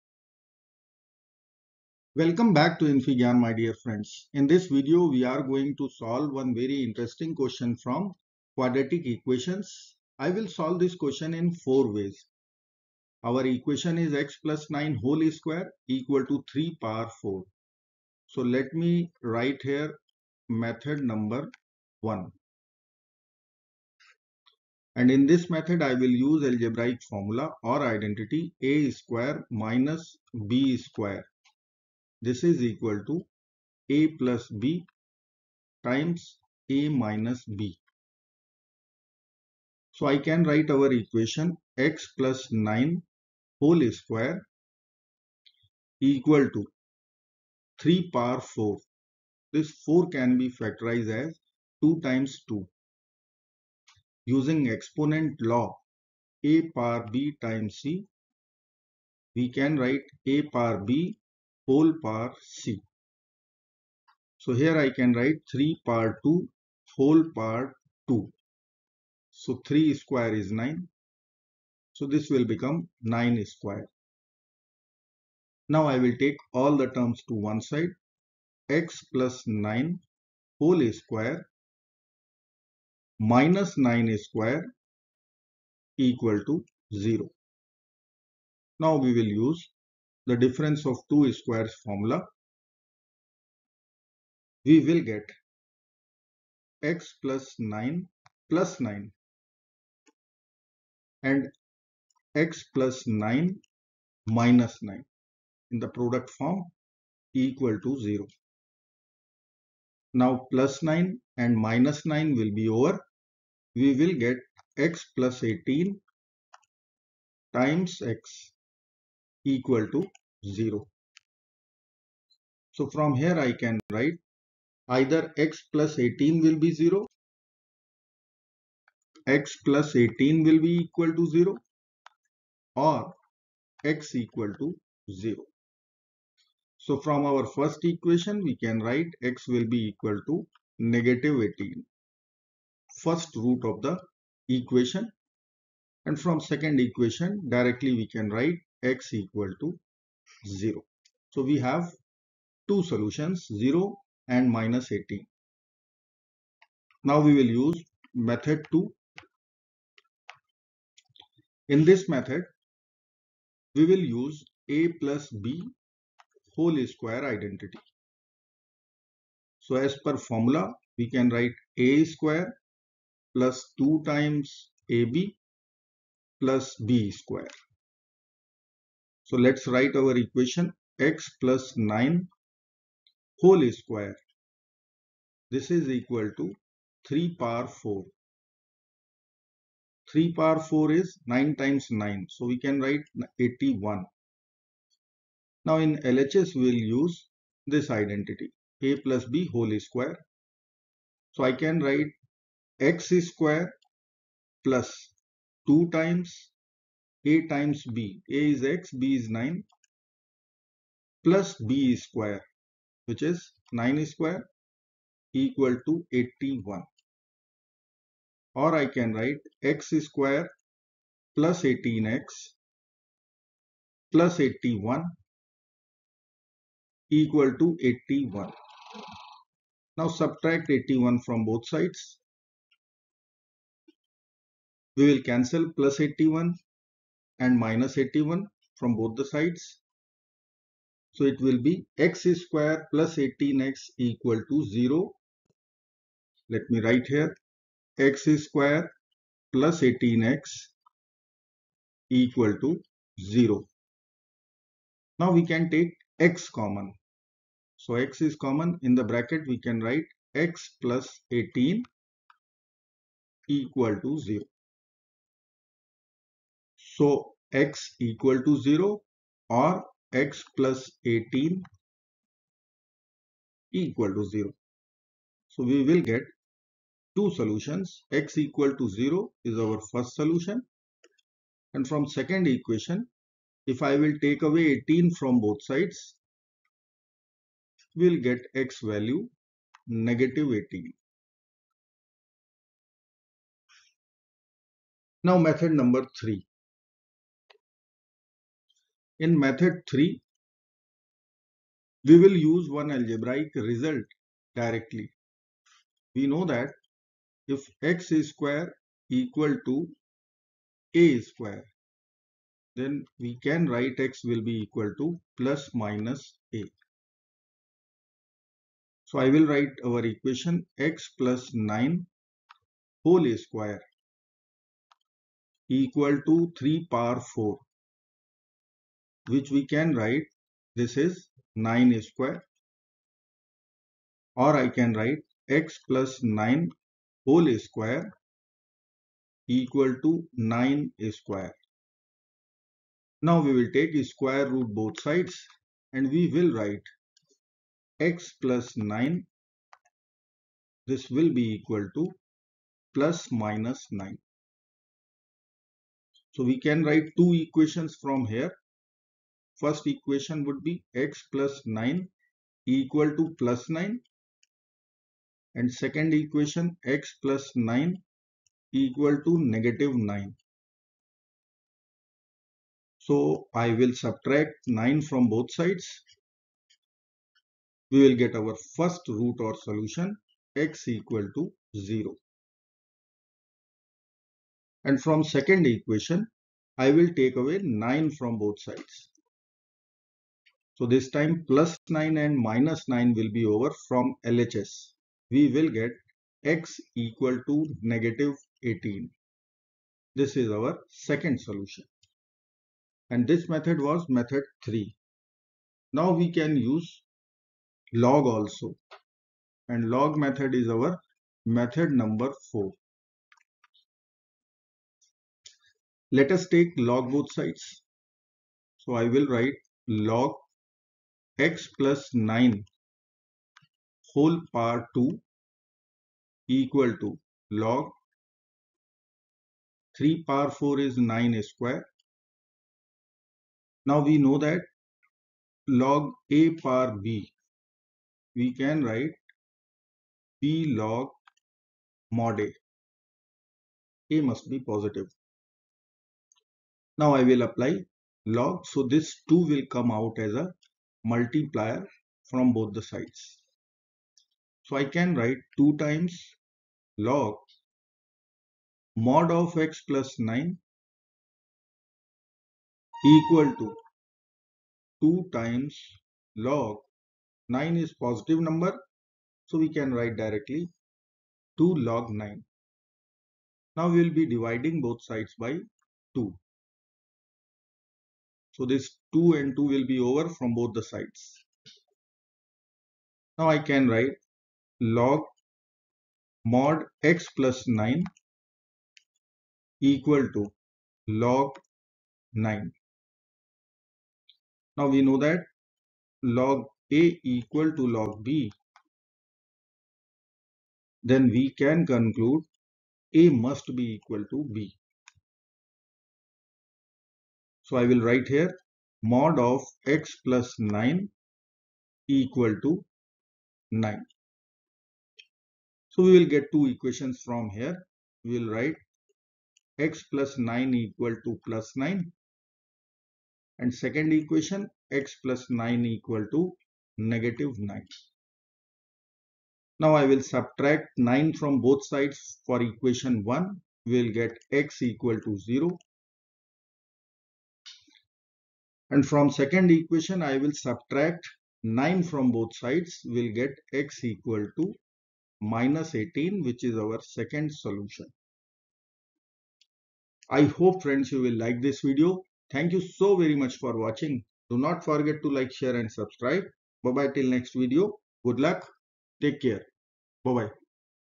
Can Algebra Be EASY With These 4 Simple Tricks?
Ready for a mathematical adventure? Join us in the Algebraic equation challenge, where we unravel 4 efficient and easy solutions for success! 🚀 Whether you're a student, enthusiast, or problem-solving aficionado, this video breaks down quadratic equations in a way that's easy to grasp and apply. 🧠💡 Get ready to conquer the challenge!

📚 Topics covered:

Introduction to Quadratic Equations
Quick Techniques (algebraic identities and logarithms) for Solutions
Converting into linear equation
Absolute value , exponent laws, properties of logarithm
Pro tips for Success

🎓 Level: Beginner to Intermediate